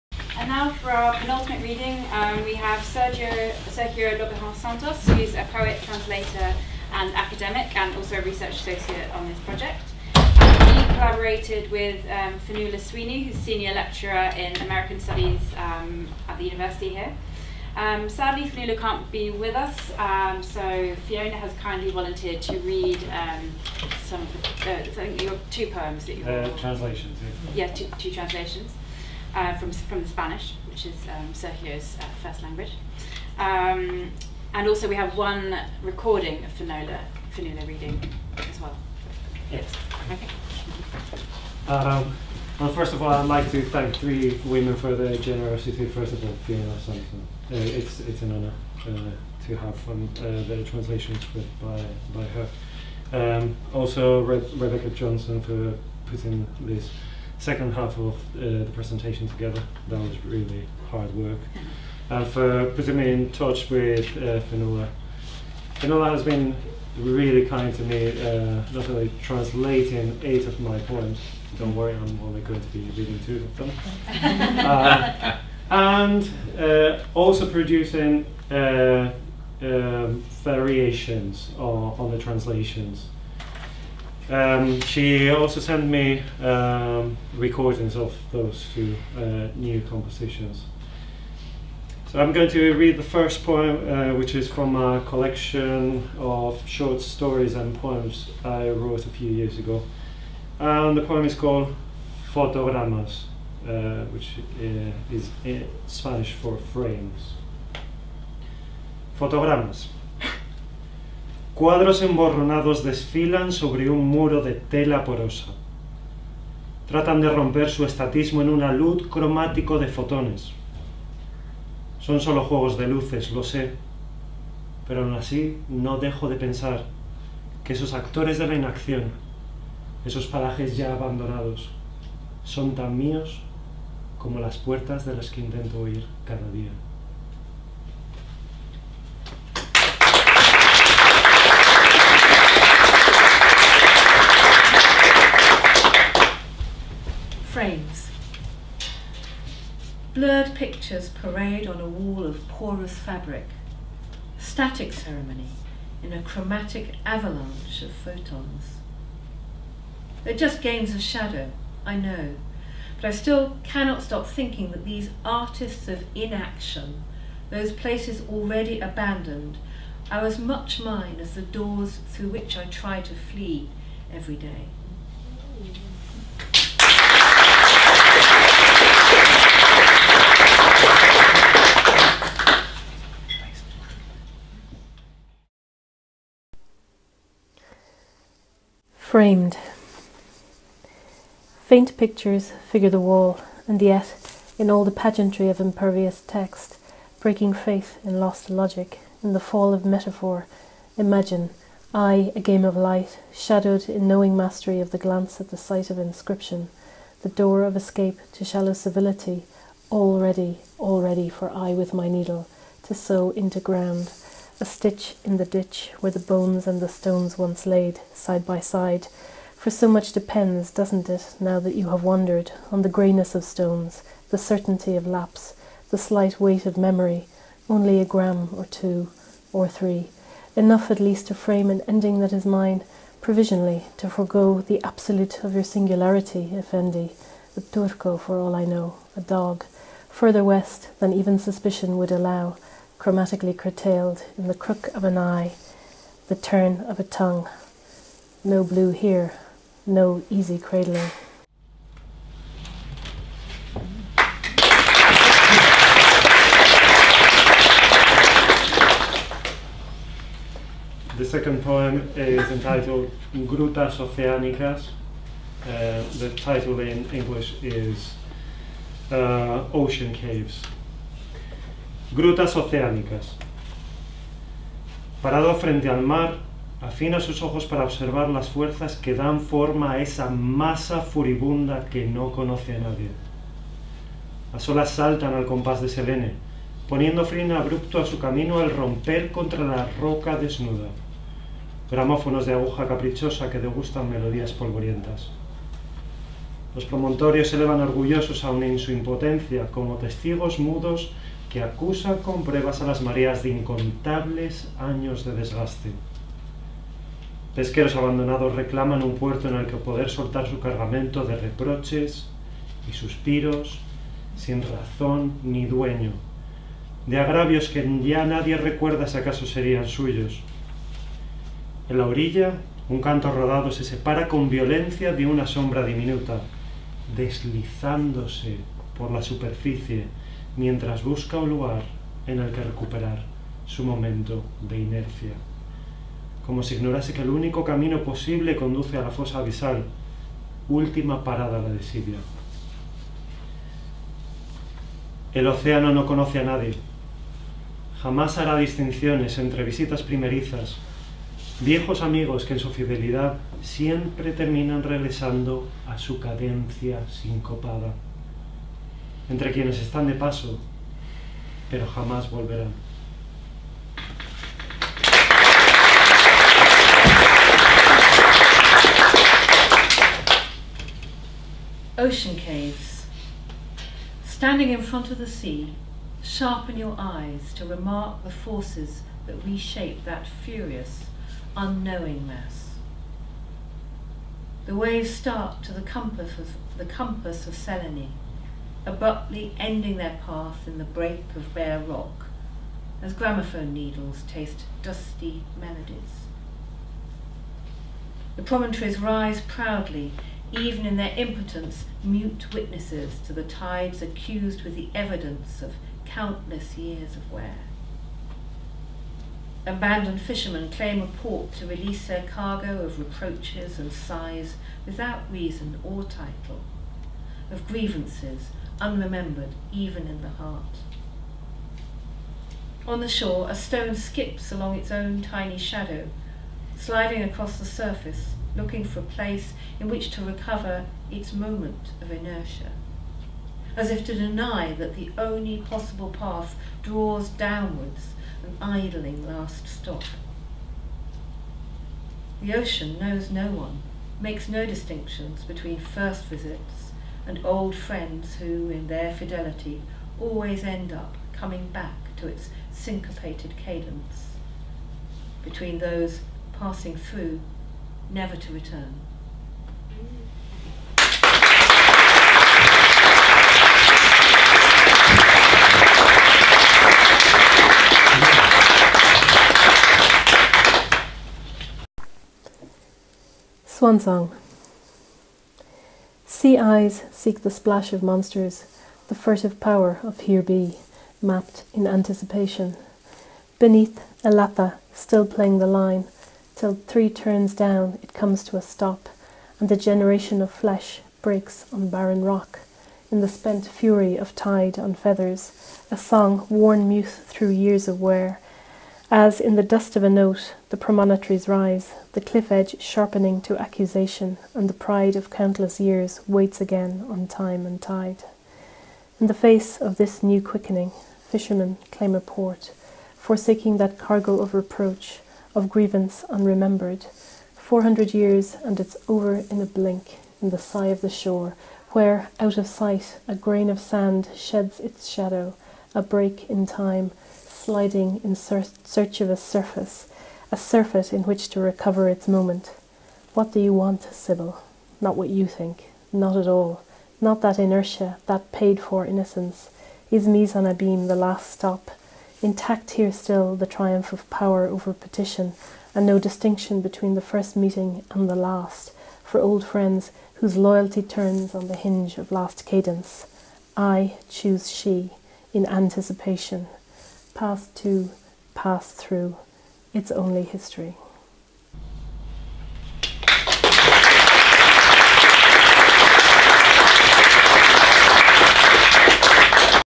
Sergio Lobejón Santos and Fionnghuala Sweeney - Poettrio Collaborative Translation Event
Presentation of the original poetry and translations produced by Sergio Lobejón Santos and Fionnghuala Sweeney at the Collaborative Translation event organised by the Poettrio Experiment, featuring readings by Fiona Sampson.

You can find a link to the poems and translations, as well as the authors' comments about the collaborative process on the Poettrio

Recorded on 20 July 2017, Percy Building, Newcastle University.